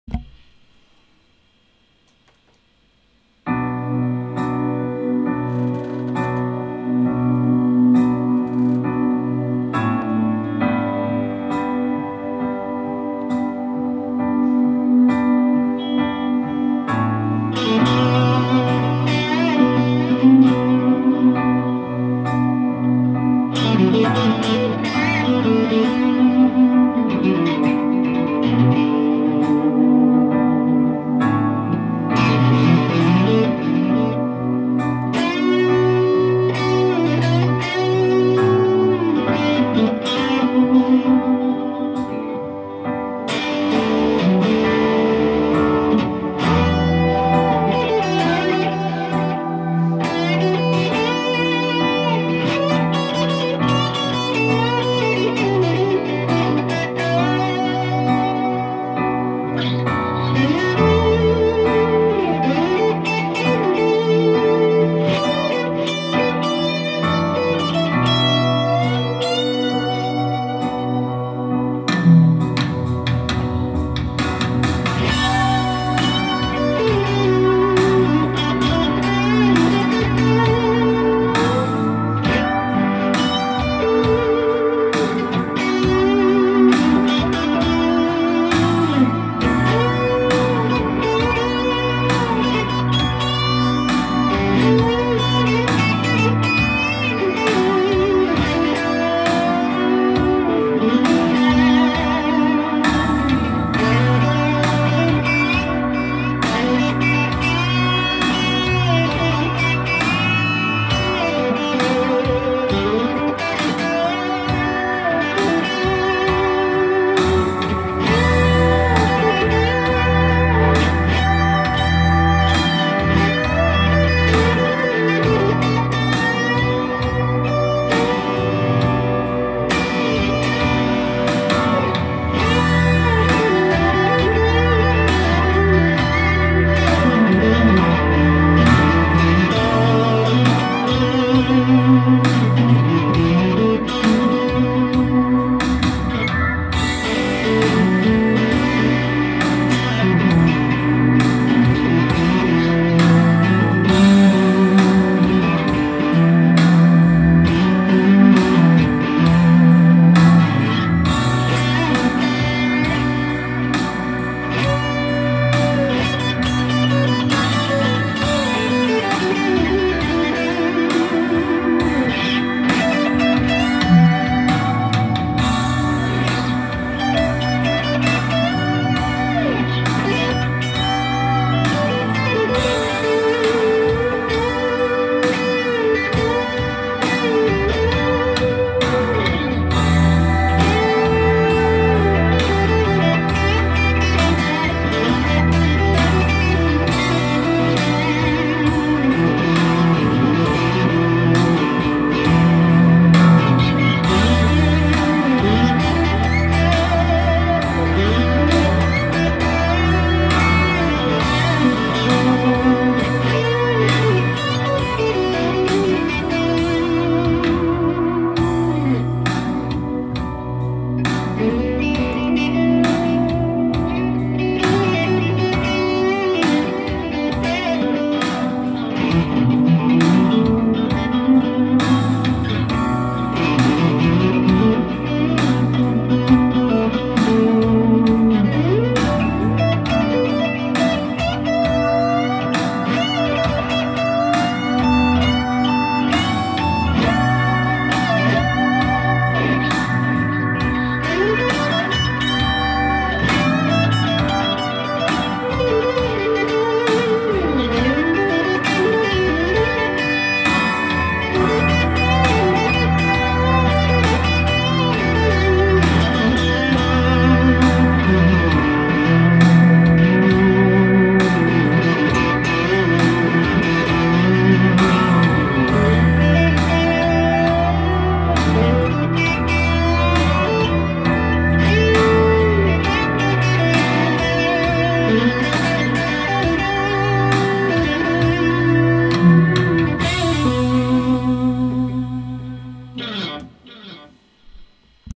2010 Fender Roadworn Telecaster/Texas Special PU`s
Marshall Vintage Modern Amp/MXR-Booster/VOX Time - Machine Delay.Thanx to Vito Astone for the great backing track.